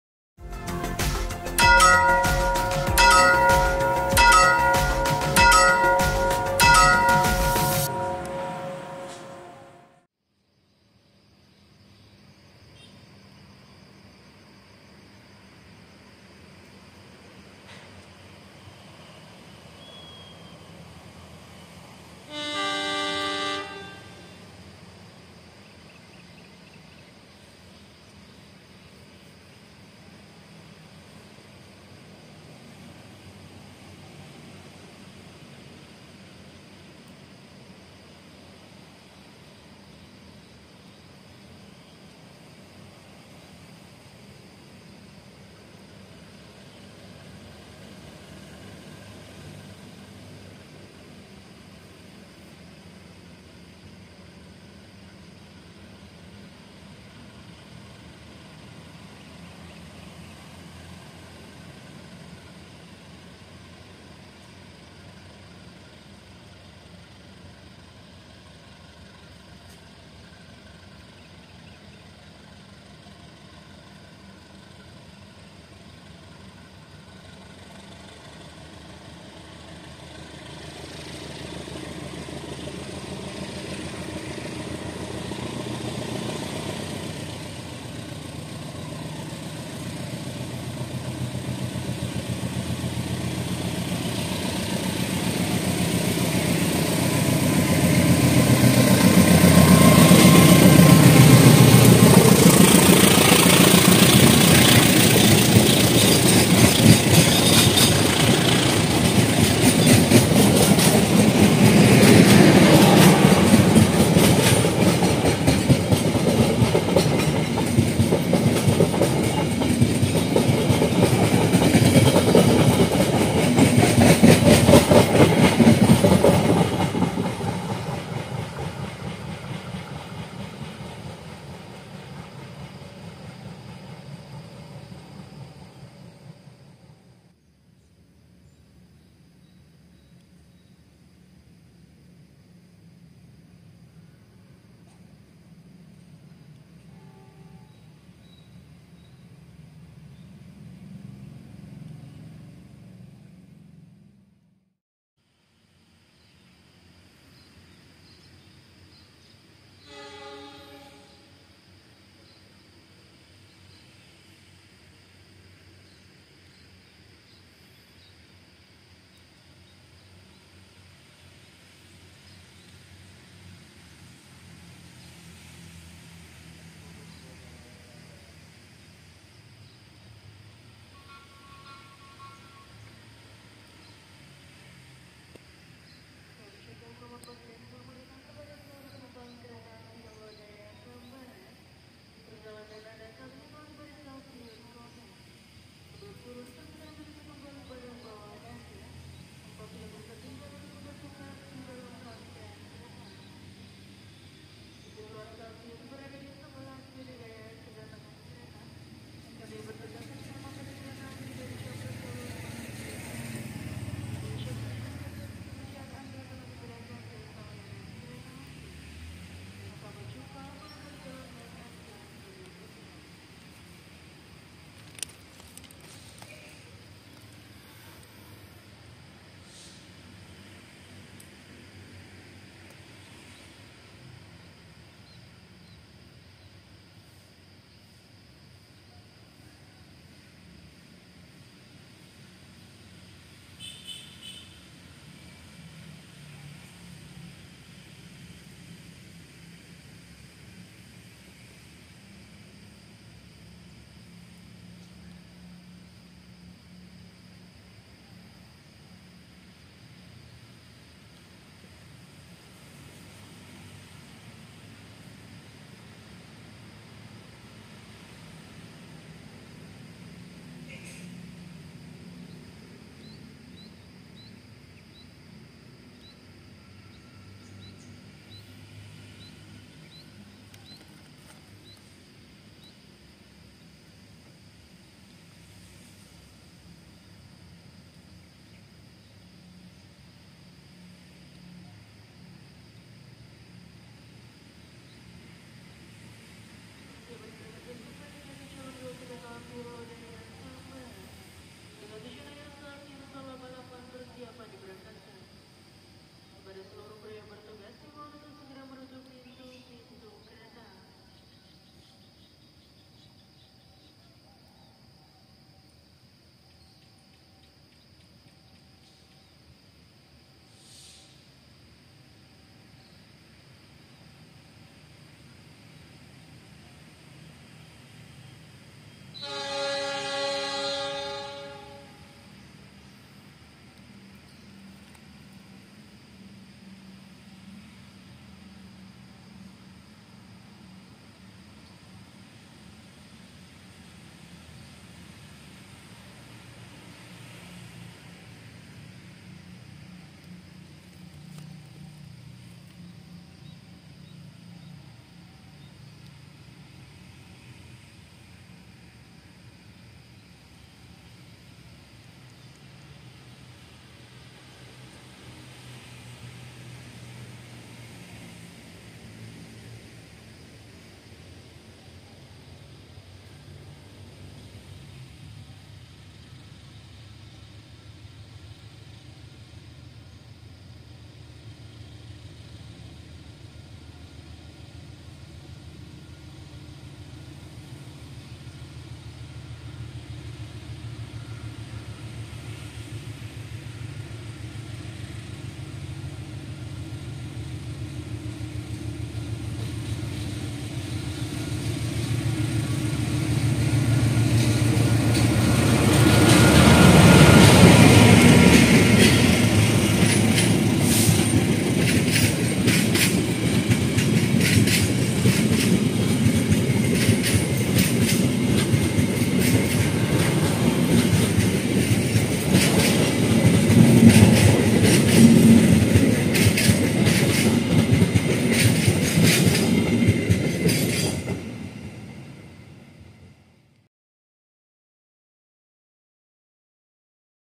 Hunting Pagi (Lagi) di Timur Depo KRL Klaten
Hunting pagi lagi di timur Depo KRL Klaten, mepet di perumahan warga.
Tempat yang enak untuk hunting walaupun tidak ada tempat yang layak buat duduk :-)
Itu sebabnya saya tidak betah berlama-lama di sini, apalagi ini adalah pemukiman warga...